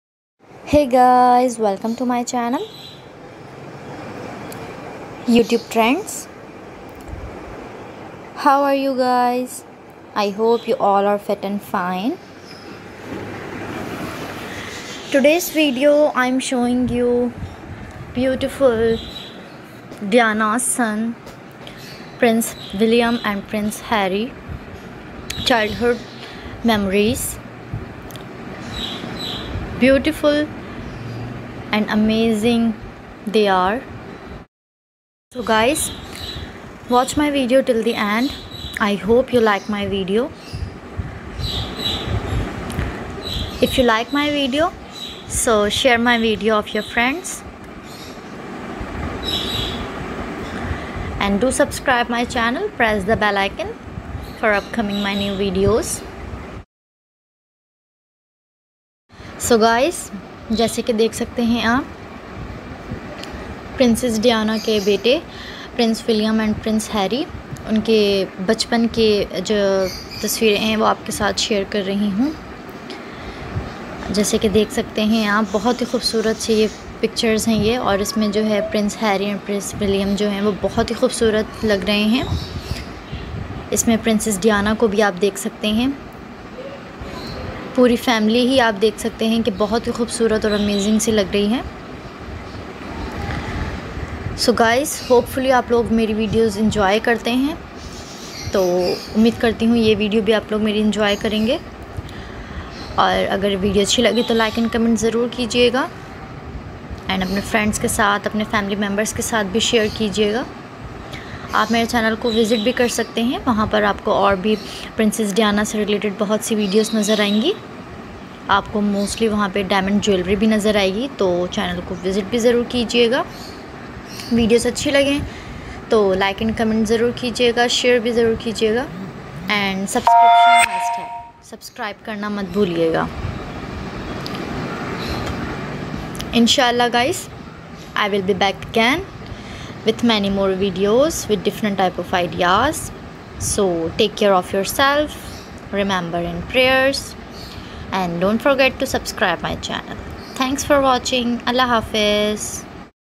30 Adorable Childhood Memories of Princess Diana son's, Prince William & Prince Harry
How Sweet! Prince William and his younger brother, Prince Harry, had the Cutest childhood photos...